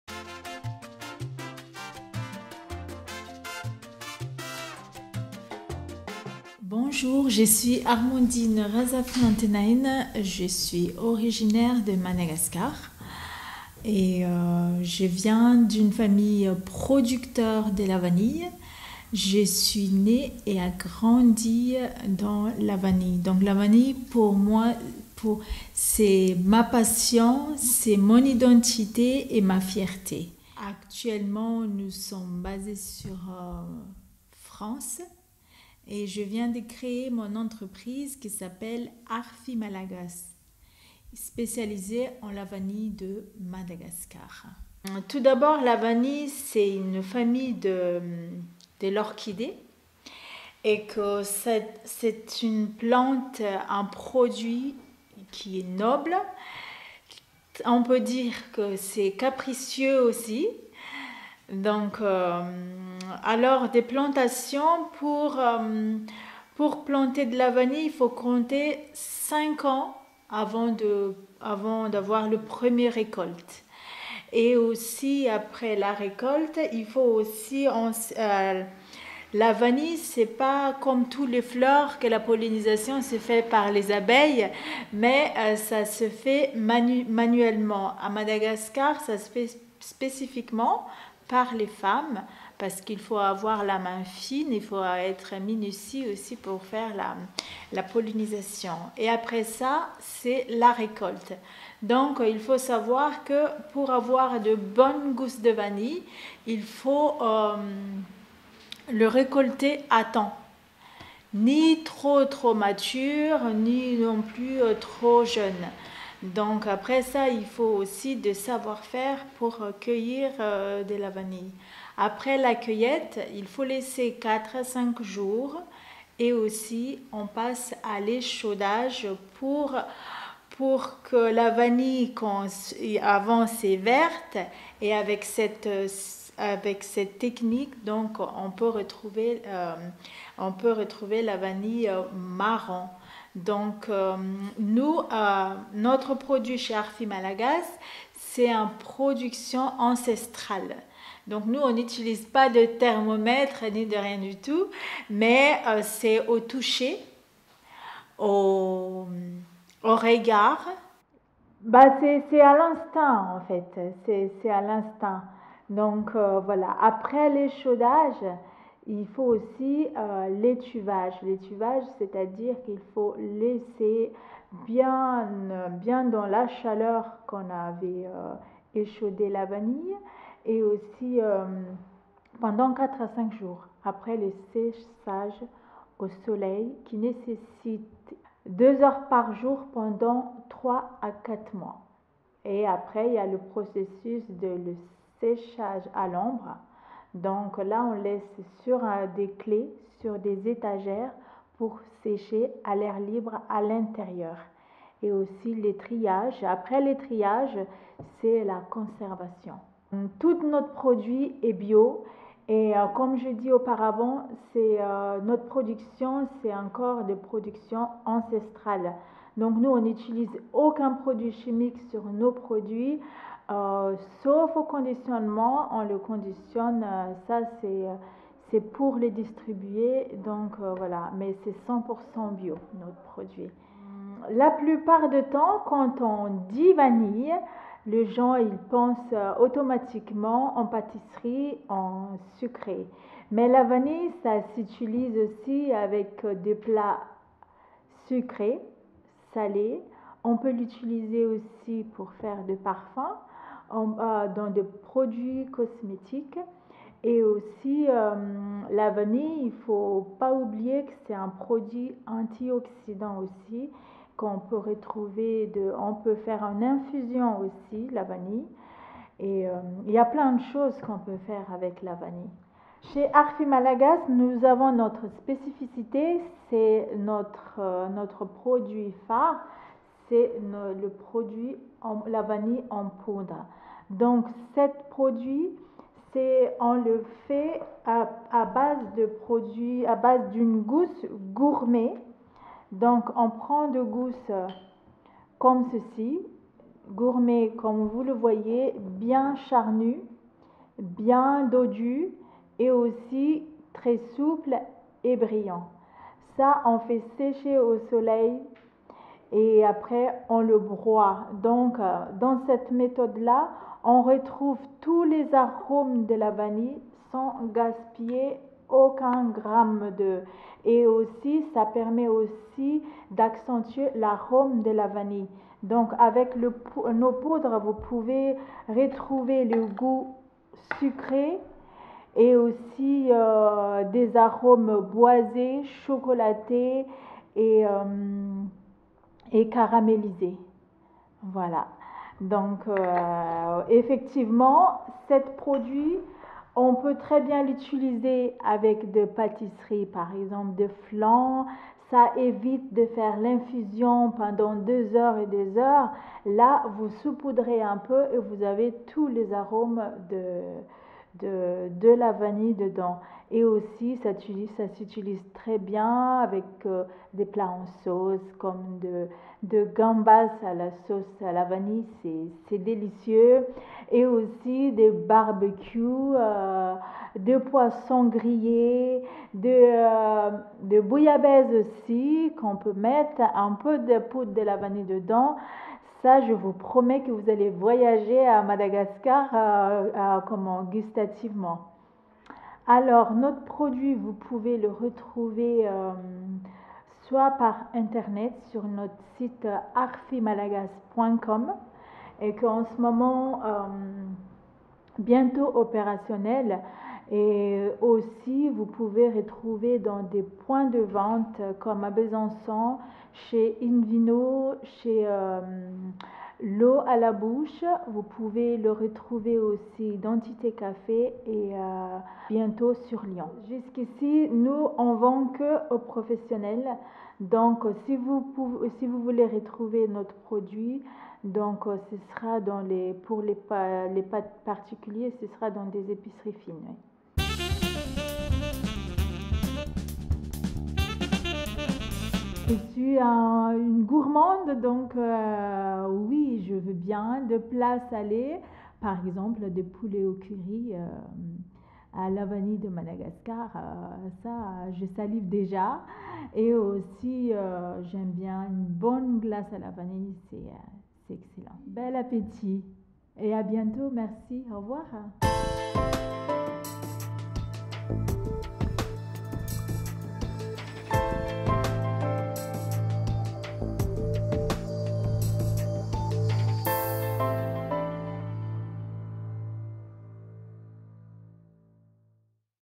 Vanille Madagascar by ART-FY MALAGASY
La Vanille de Madagascar by ART-FY MALAGASY
Originaire de Madagascar, où sa famille a implanté la culture de la vanille Bourbon (de Madagascar), Armondine Razafinantenaina est désormais installée en France.
Il y a créé sa société ART-FY MALAGASY qui importe cette fameuse et exceptionnelle vanille de Madagascar, en gousse comme en poudre.
Actuellement, son commerce est réservé aux professionnels de la restauration, mais on trouvera bientôt cette épice en boutique spécialisée avant la fin de l'année 2021.
Armondine est interviewée par Michel Godet, rédacteur en chef de Rabelais.TV